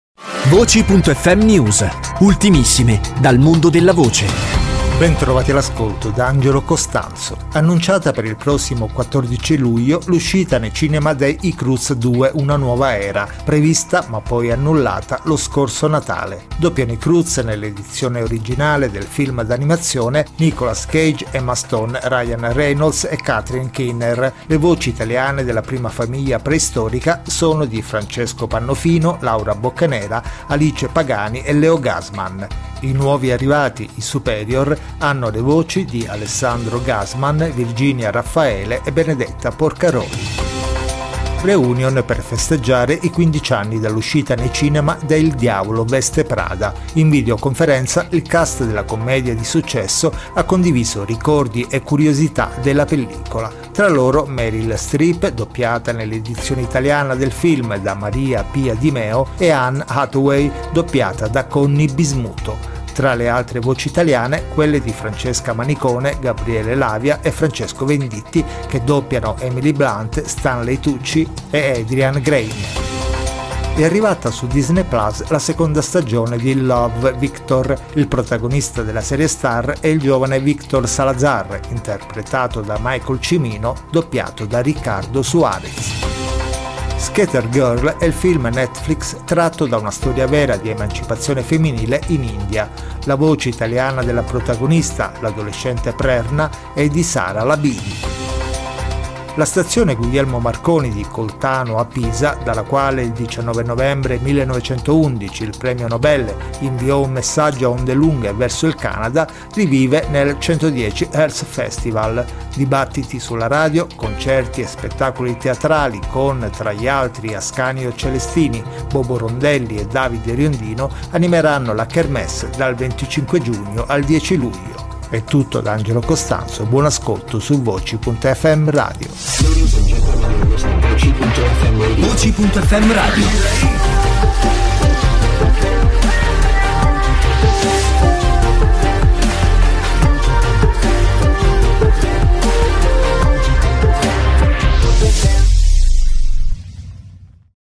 NEWS DAL MONDO DELLA VOCE (18-06-2021)
Nuova data per l'arrivo al cinema del sequel d'animazione "I Croods 2: Una Nuova Era"; al via a Pisa nel segno della radio e di Guglielmo Marconi il "110 Hertz Festival" e molto altro dal mondo della radio e del doppiaggio.